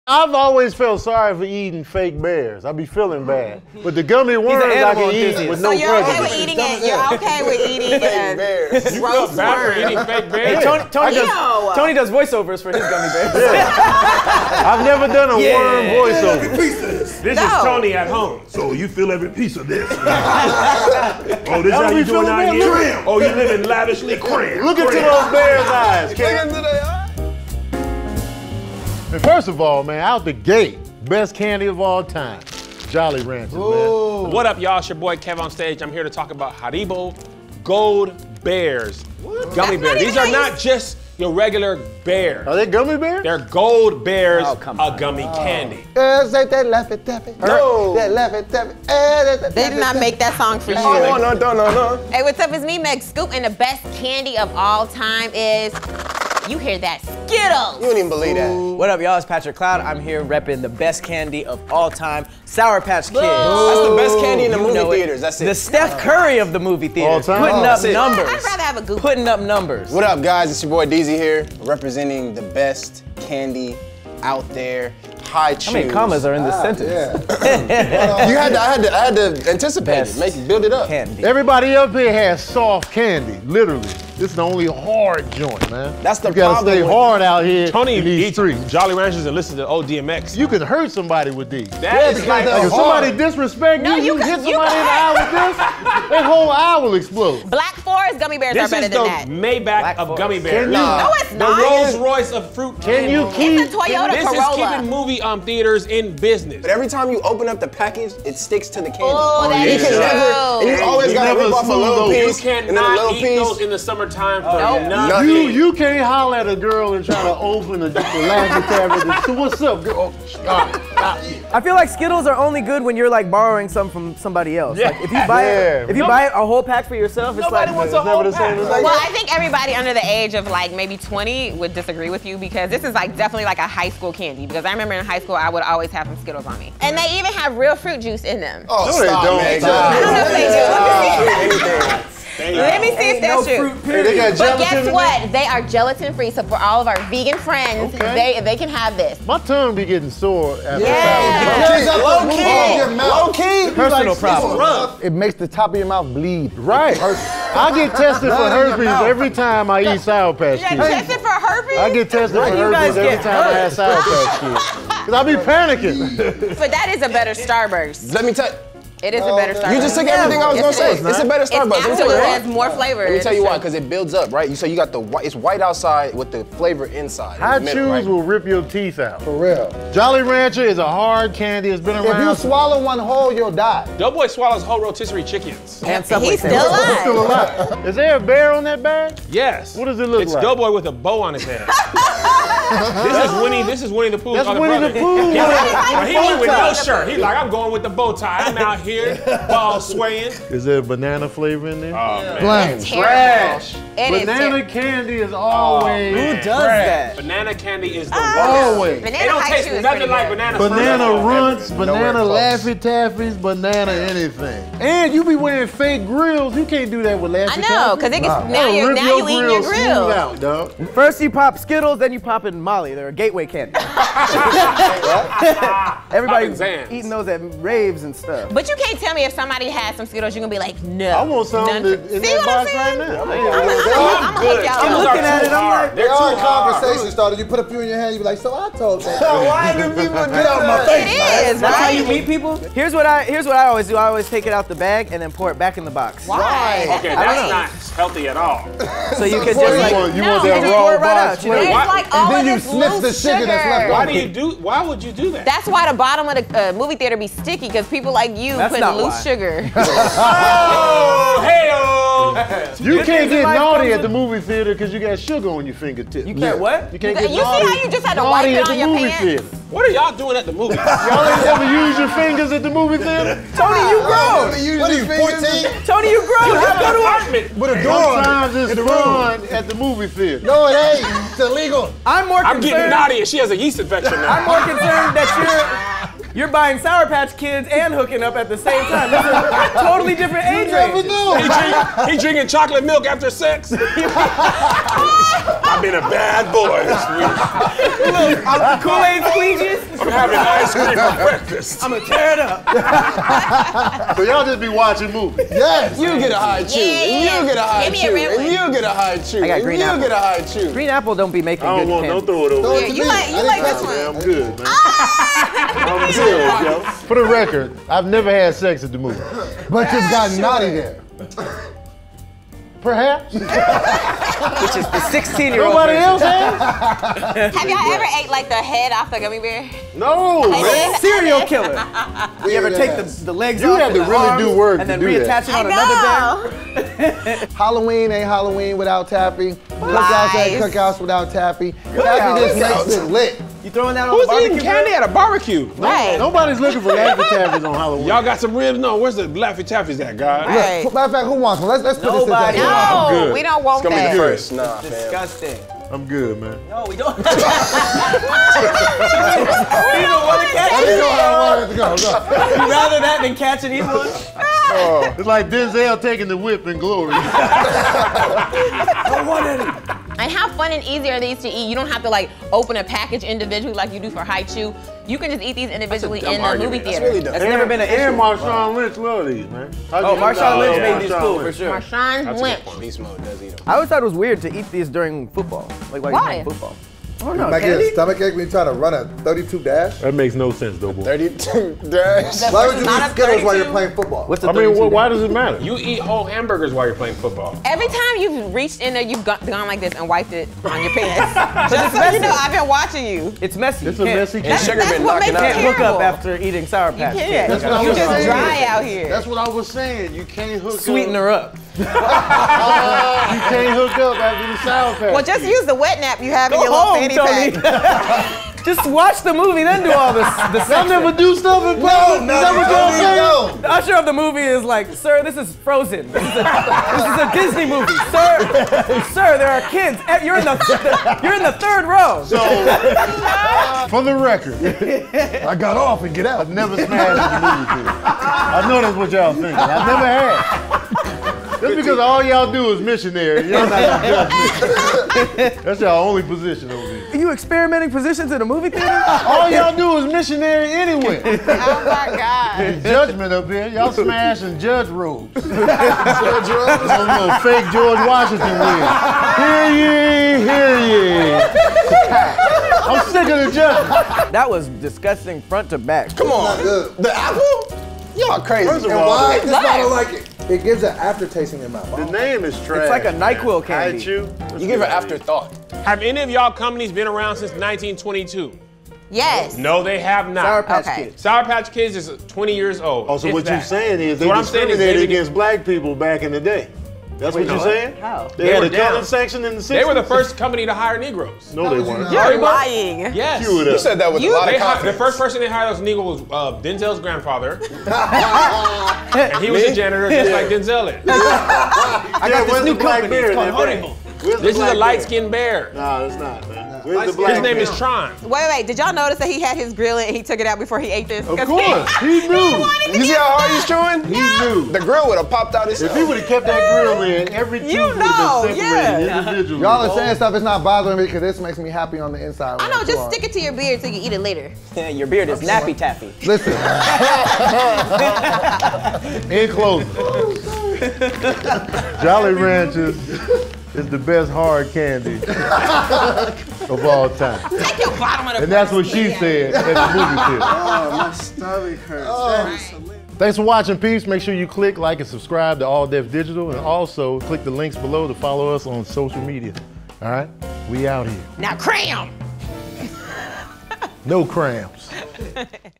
The Best Candy | Great Taste | All Def
The SquADD discusses their all-time favorite candy. Which candy is the best?

[CREDITS]

Starring: @iamdoboy @kevonstage @megscoop @deazyyach @tonybakercomedy @patrickcloud
Producer: Tahir Moore
Director: Patrick Houston
Director of Photography: Daryl Gilmore
Camera Operators: Ben Hecth
Sound Mixer: Jacob Harrold
Production Assistant: Dylan Marx, Cedrick Larkins
Post Supervisor: Trish Pelefoti
Editor: John Norris